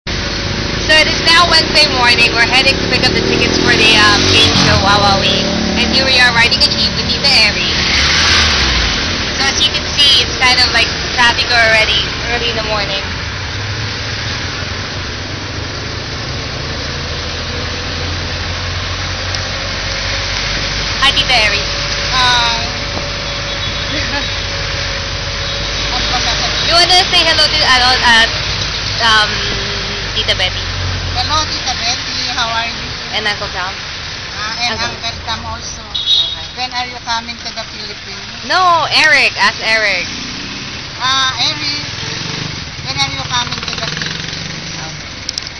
riding a Jeepney in the Quezon City with Tita Erie
She's the best Aunt ever.  Taking me around while on vacation.  Nov 2008